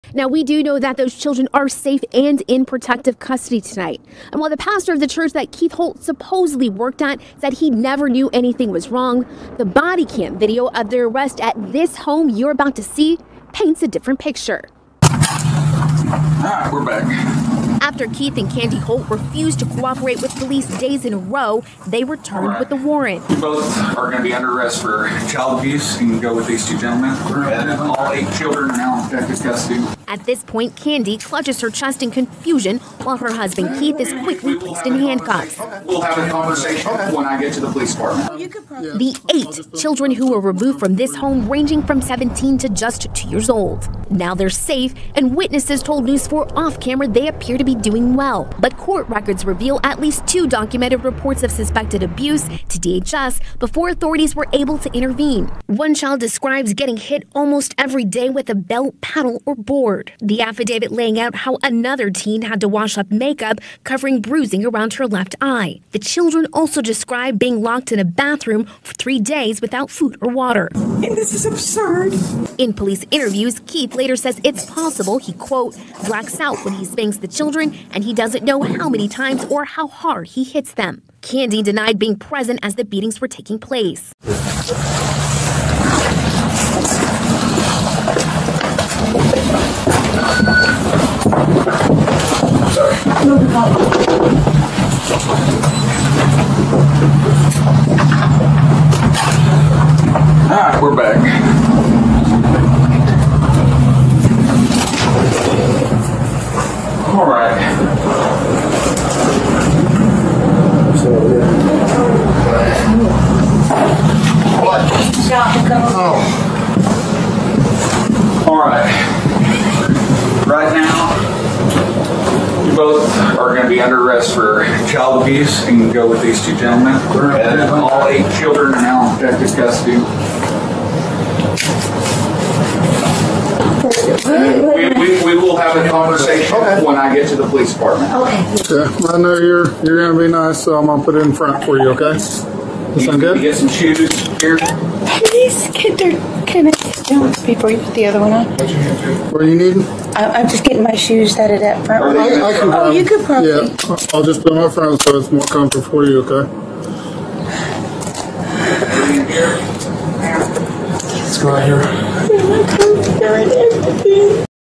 Oklahoma Pastor & wife arrested for alleged kidnapping and child abuse
A pastor and his wife were arrested following serious allegations of child abuse, kidnapping and neglect.

Body-worn camera footage provided to KFOR documents the moments they were taken into custody at their home.

An incident report from Blackwell Oklahoma Police shows authorities had been looking into Keith and Candy Holt since the beginning of January, after a complaint was made to DHS that the couple left several children alone and traveled out of state.

A statement provided by Chief J.Brewer, Blackwell Police Department, notes several attempts to get in contact with the couple at their home:

January 14 – Blackwell Police Department and DHS went to the residence to check on the children; the parents refused to open the door

January 15 – Blackwell Police Department and DHS made another attempt; the parents still refused to open the door

January 16 – Blackwell Police Department and DHS went inside the house; a short investigation produces enough probable cause to arrest both parents for child abuse

Eight children under the age of 18 were removed from the home and placed in child protective custody.

In court documents obtained by KFOR, Keith Holt is accused of beating the children with a board, and in some cases, locking them inside a bathroom with no food or water; his wife Candy is accused of allowing it to happen

KFOR